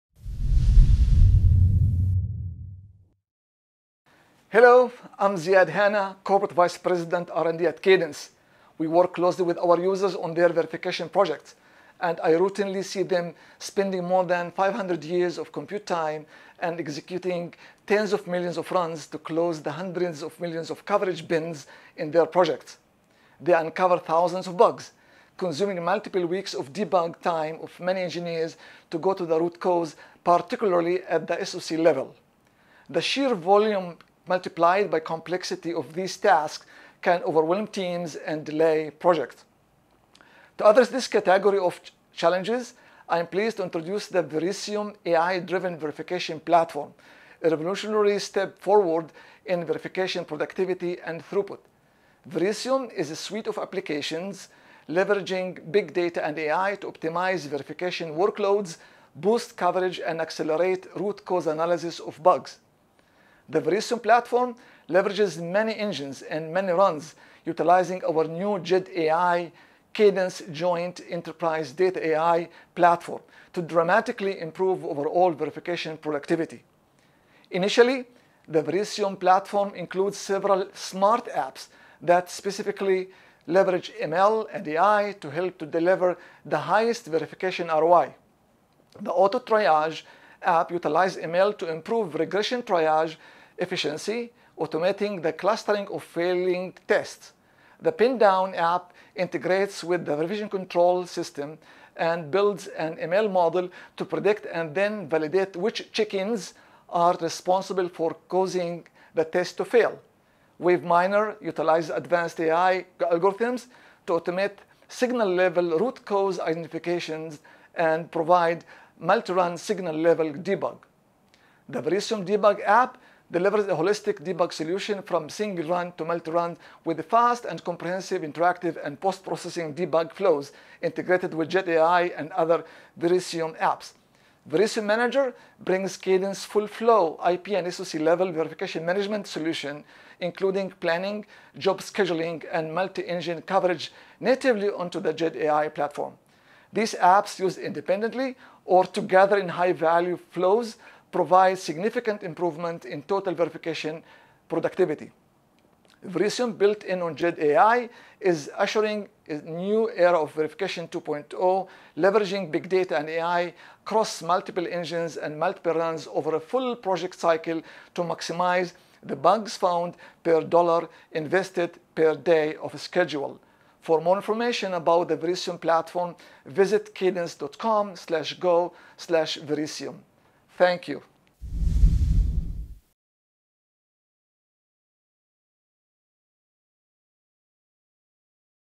Introducing the Verisium AI-Driven Verification Platform
The Verisium AI-Driven Verification Platform is a revolutionary step forward in verification productivity and throughput. Learn how the Verisium platform’s suite of applications leverages big data and AI to optimize verification workloads, boost coverage, and accelerate root cause analysis of bugs.